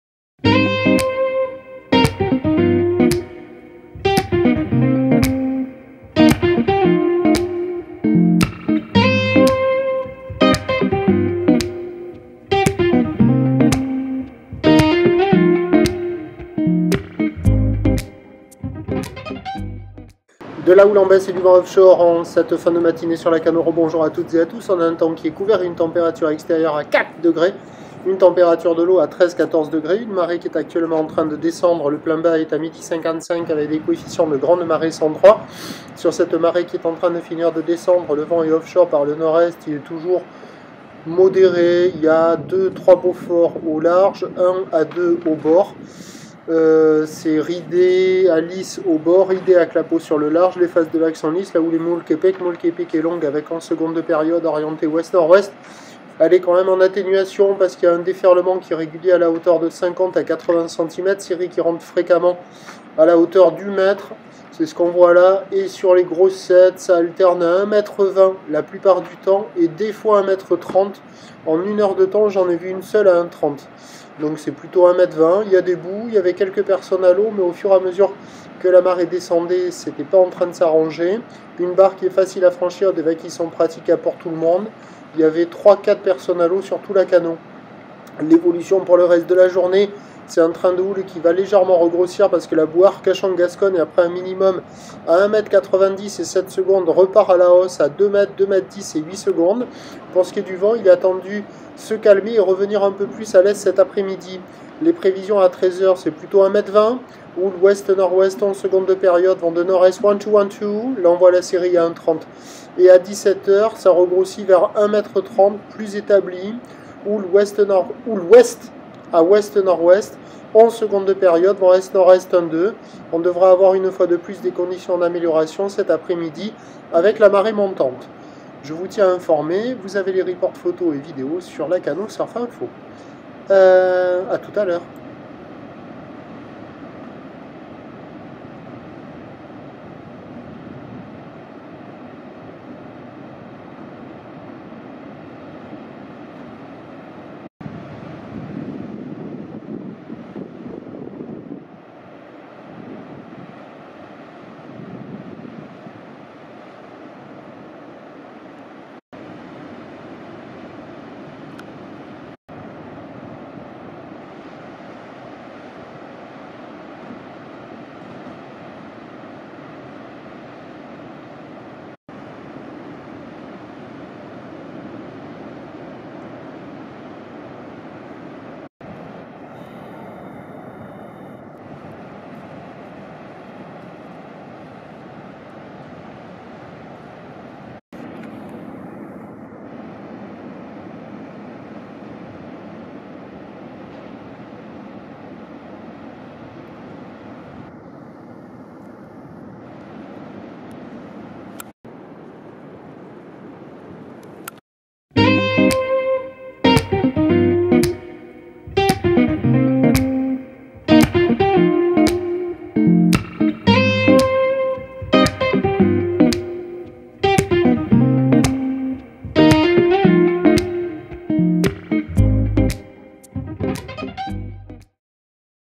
Lacanau Surf Report - Mardi 24 Janvier 11H
Houle longue ONO en baisse, vent offshore modéré.